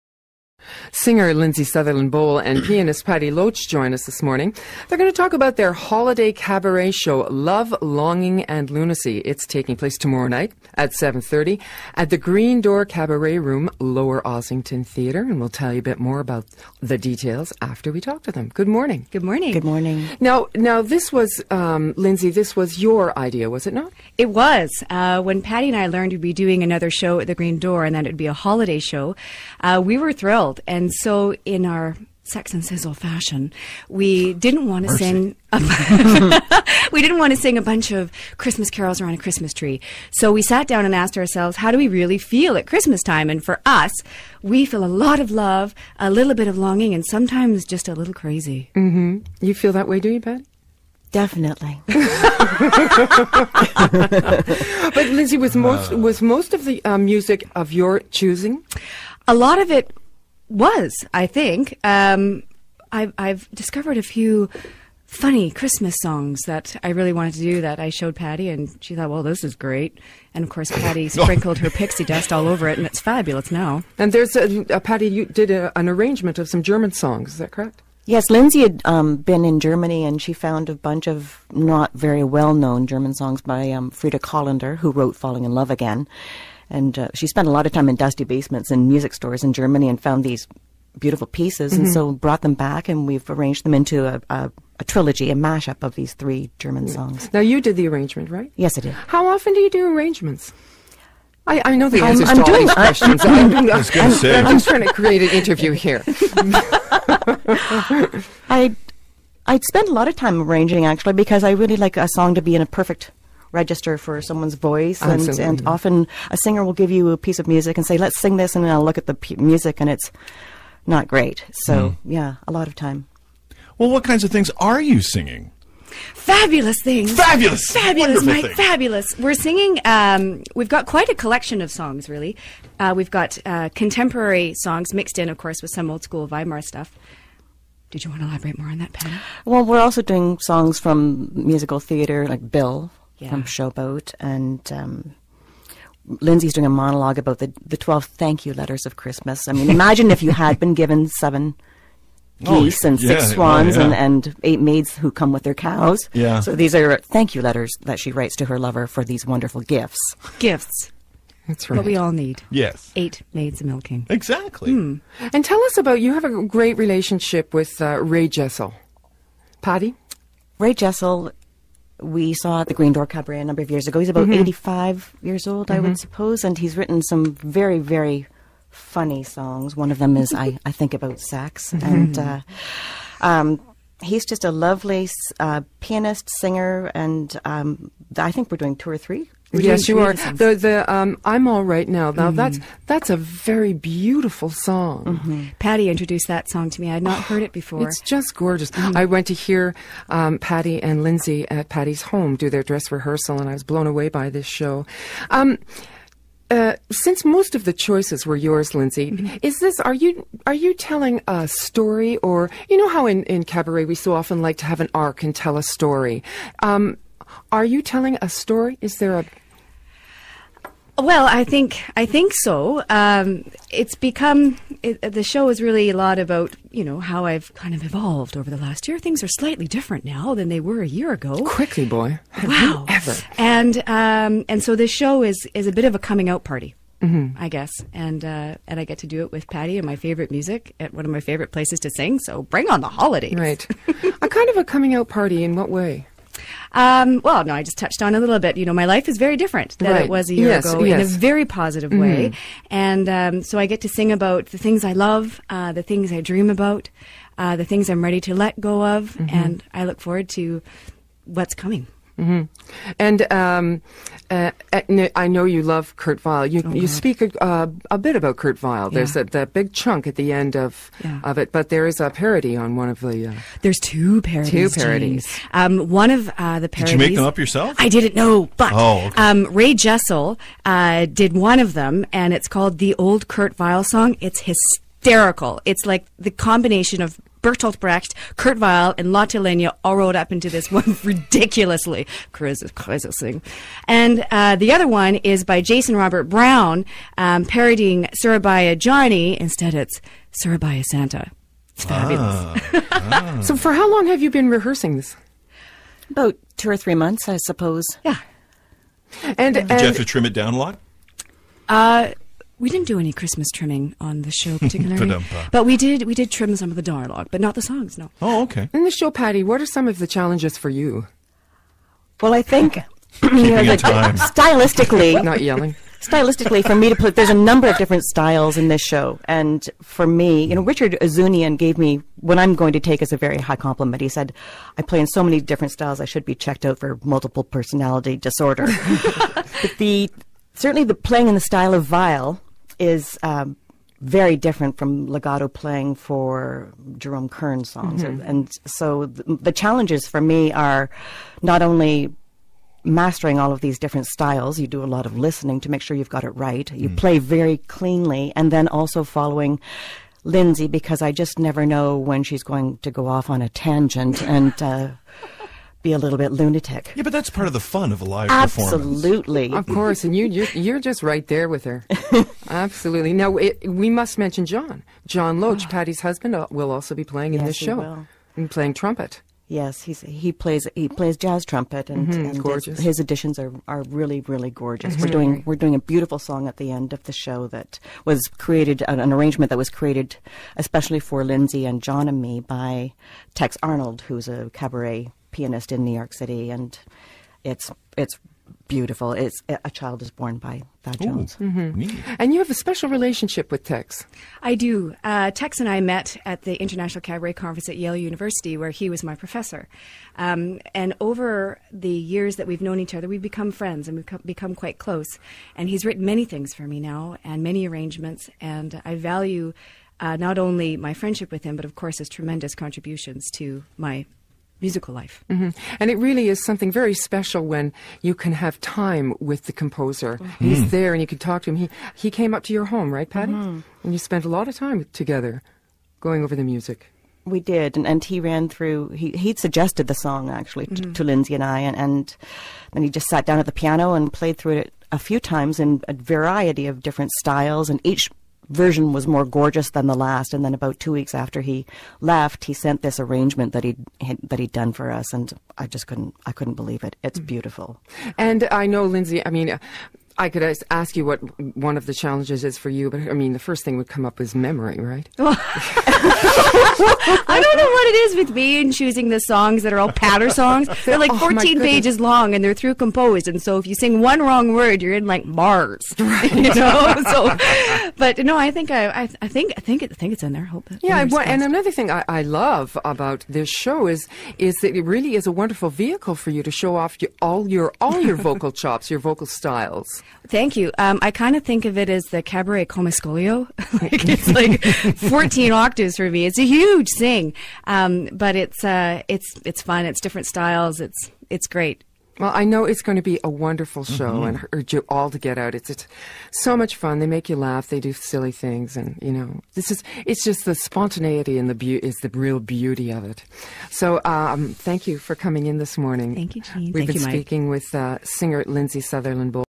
Jean Stilwell and Mike Duncan interview Lindsay Sutherland Boal and Patti Loach at Classical 96.3FM
Lindsay Sutherland Boal and Patti Loach chat about their new show, "Love, Longing and Lunacy: a Holiday Cabaret" with Jean Stilwell and Mike Duncan at The New Classical 96.3FM, Toronto.